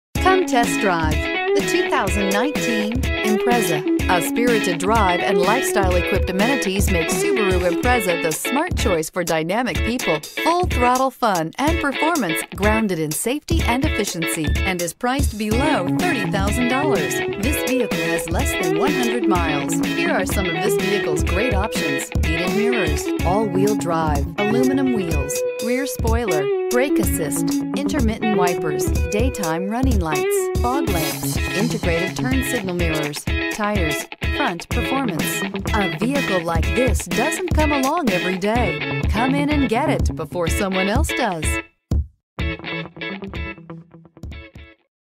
2019 Subaru Impreza 192052
2019 SUBARU IMPREZA Parsippany, NJ
Stock# 192052

Paul Miller Subaru
3469 Route 46 E

BACK-UP CAMERA, ALL WHEEL DRIVE, LEATHER, HEATED FRONT SEATS, LANE DEPARTURE WARNING, BLUETOOTH, MP3 Player, KEYLESS ENTRY, 36 MPG Highway, SAT RADIO, ALLOY WHEELS.